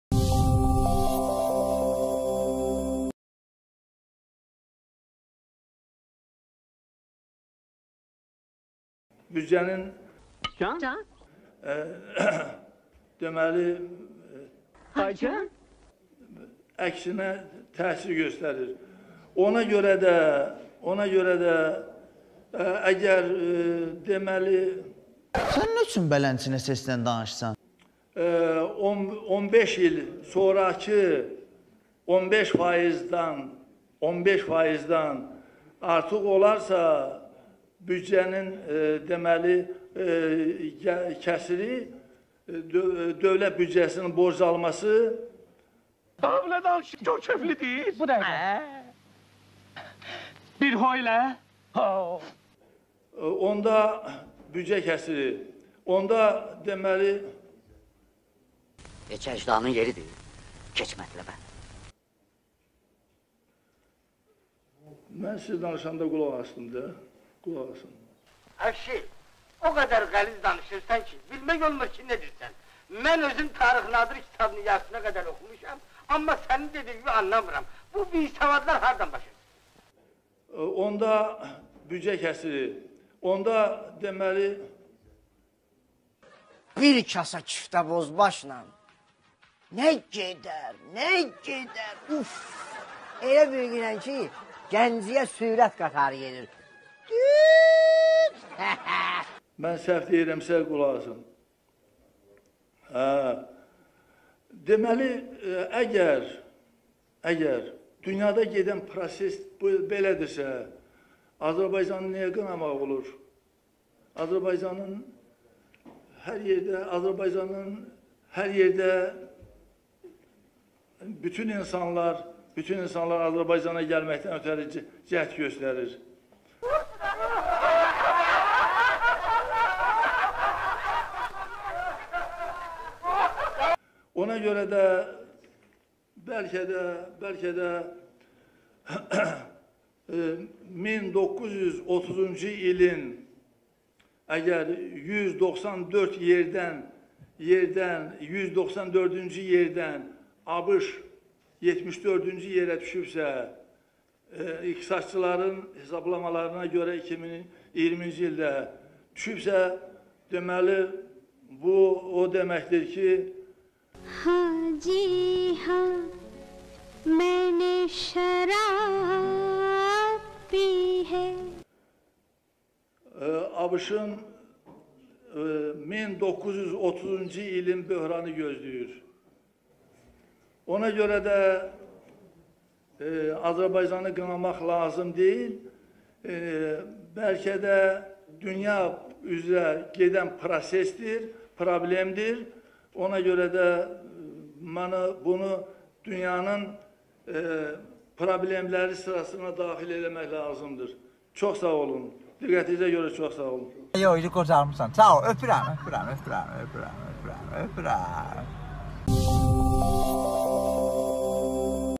77 yaşlı deputatın çıxışına satirik baxış-BİR DƏ BELƏ İZLƏYİN!
Əldə etdiyimiz məlumatlara görə 77 yaşlı deputat Xanhüseyn Kazımlının səhhətində problemlər var...Kanal 13 olaraq deputata Allahdan şəfa və can sağlığı arzu edirik!